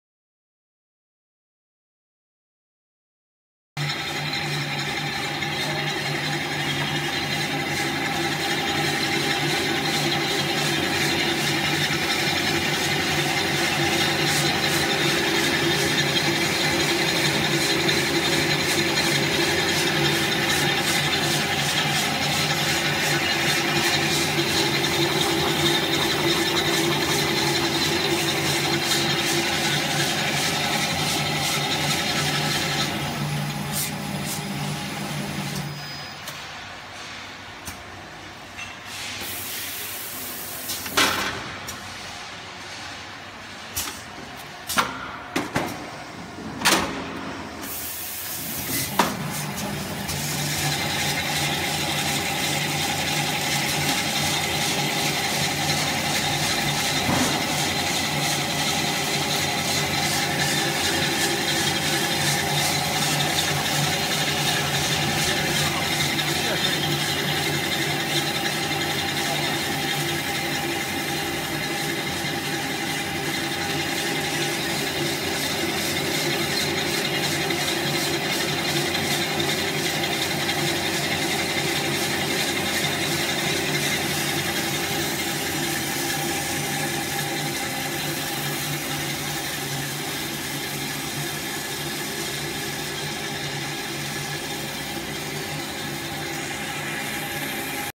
Full automatic Pre stretch film rewinding machine, USA or Dubai Pre stretch film rewinder machine
Full automatic Pre stretch film rewinding machine with high speed about 1200m per minutes, USA or Dubai Pre stretch film rewinder machine can make tight or loose rewinding effects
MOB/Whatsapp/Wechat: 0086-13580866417